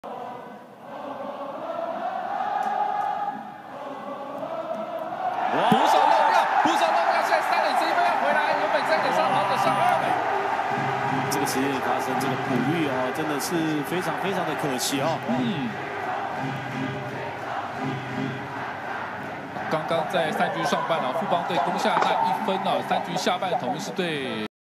2024/04/20 富邦悍將 vs 統一獅 三局下 陳仕朋暴投 三壘跑者回來 統一獅追平
轉自：中華職棒大聯盟